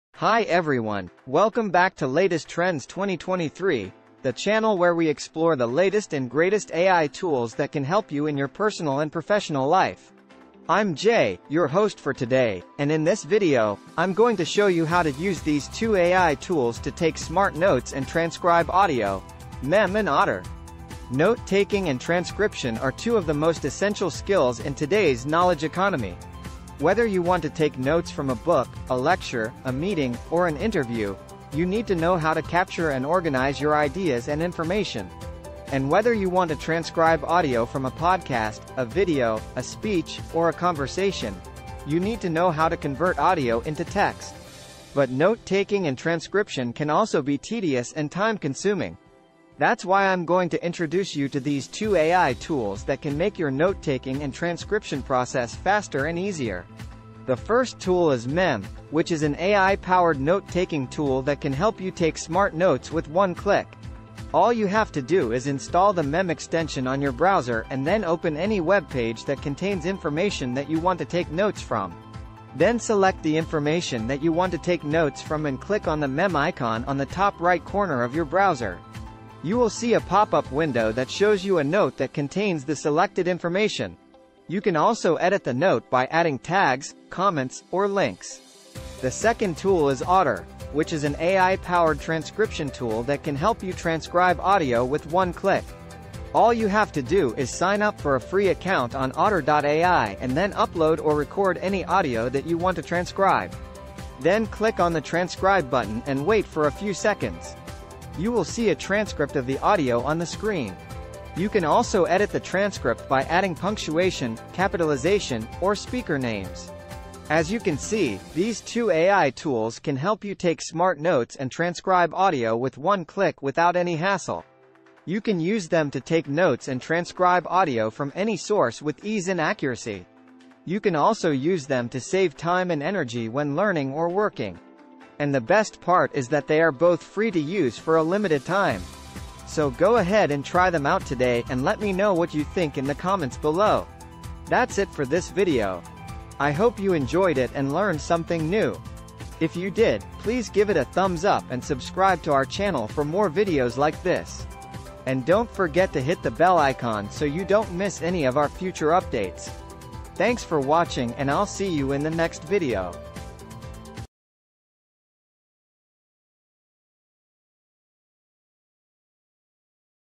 Note-Taking and Transcript Tips:How to Use These 2AI Tools to Take Smart Notes and Transcribe Audio.
In this video, you will learn how to use Mem and Otter to take smart notes and transcribe audio. You will see how these AI tools can help you capture and organize your ideas and transcribe audio from meetings, interviews, or lectures.